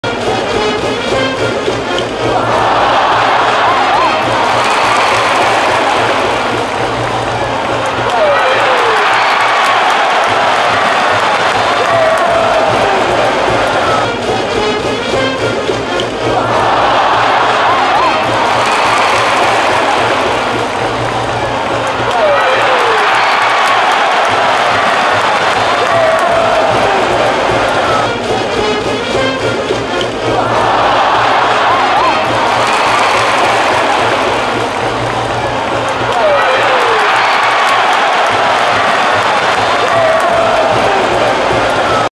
決勝戦勝負を決めた10回表2点目のタッチアップ
お互い精一杯のプレーなのでしゃーない
中継プレー出来てたらホームアウトに出来たと思う